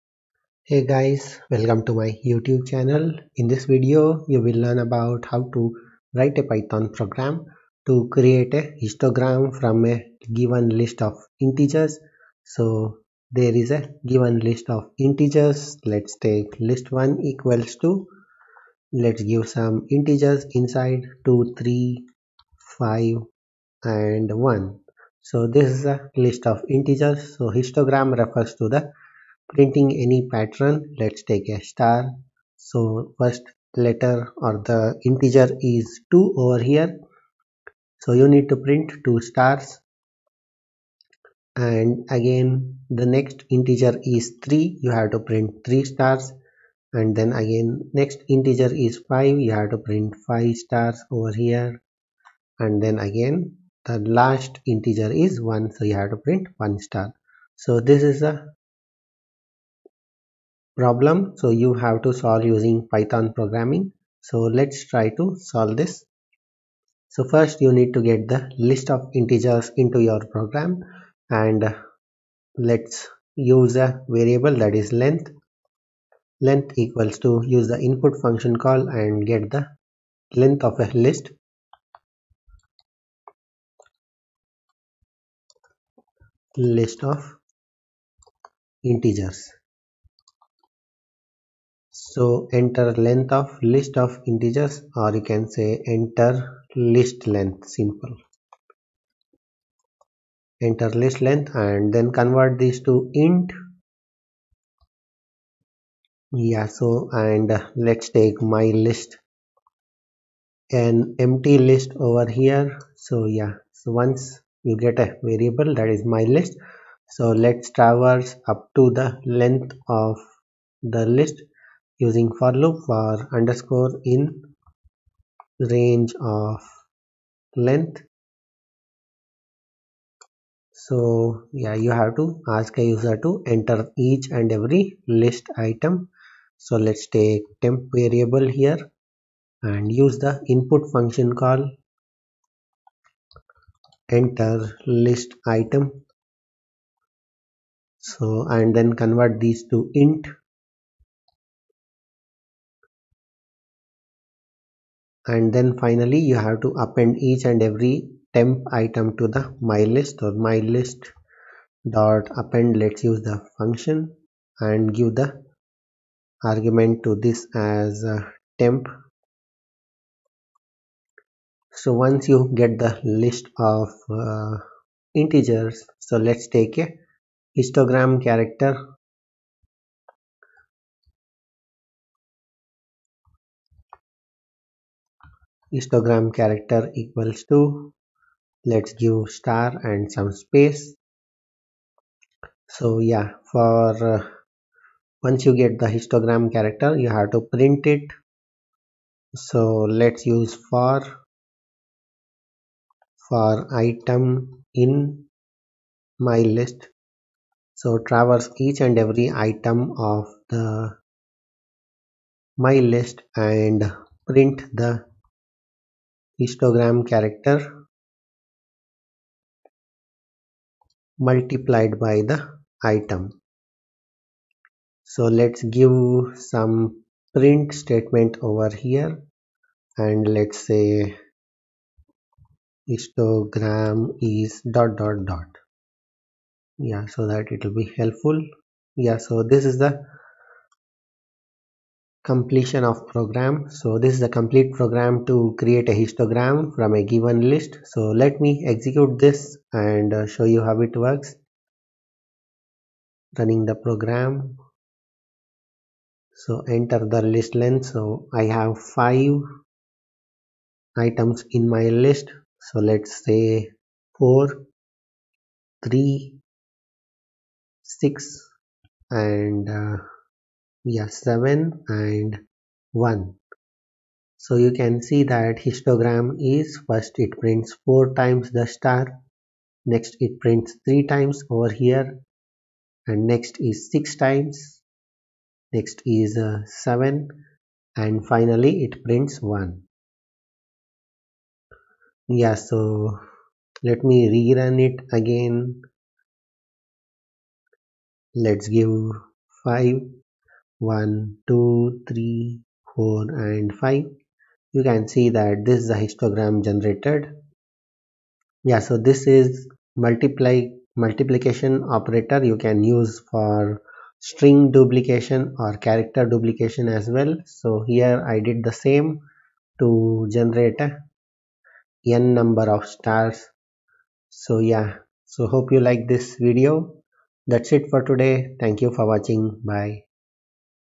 Write a Python Program to Create a Histogram From a Given List of Integers
Hi, in this video I tried to explain how to Write a Python Program to Create a Histogram From a Given List of Integers

Code
length = int(input("Enter List Length : "))
my_list = []
for _ in range(length):
    temp = int(input("Enter List Item : "))
    my_list.append(temp)

print("Histogram is ... ")
histogram_char = "* "
for item in my_list:
    print(histogram_char * item)